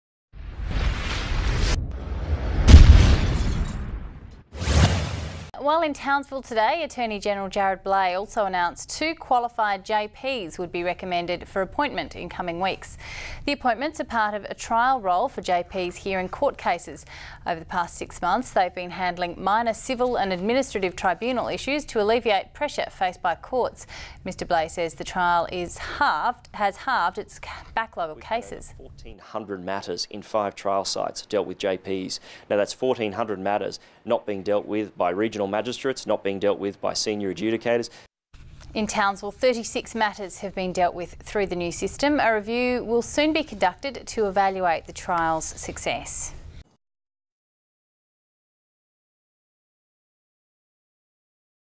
Tcad Win News - JP Trial
Queensland Attorney-General Jarrod Bleijie announces two qualified JP's will be recommended for appointment. The appointments are part of a trial role for JP's to hear court cases.